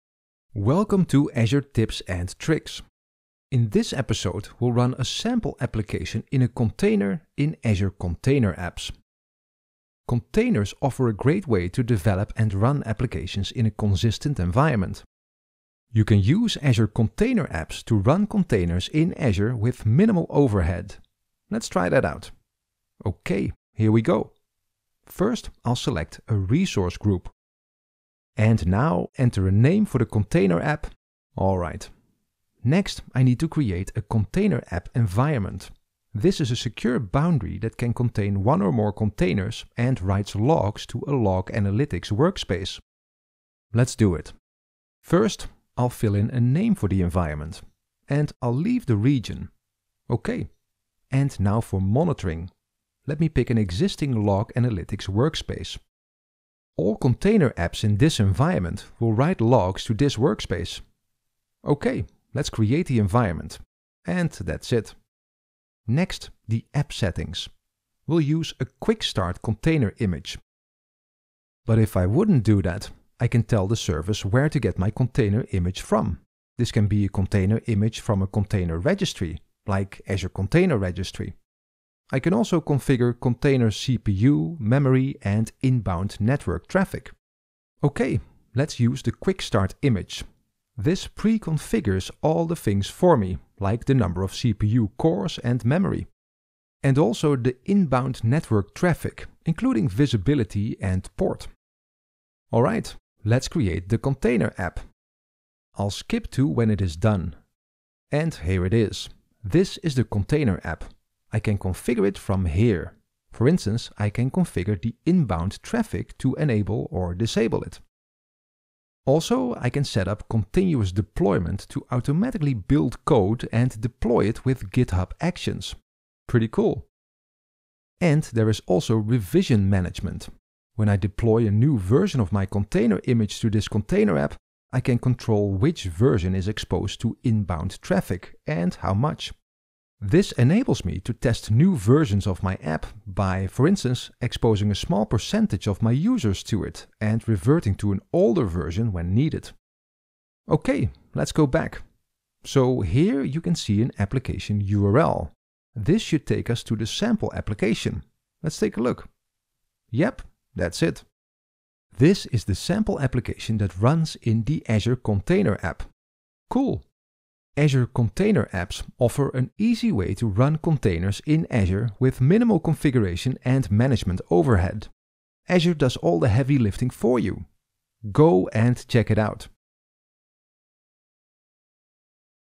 How to get started with Azure Container Apps
In this edition of Azure Tips and Tricks, learn how to get started with Azure Container Apps. Containers offer a great way to develop and run applications in a consistent environment.

Get started with 12 months of free services and $200 USD in credit. Create your free account today with Microsoft Azure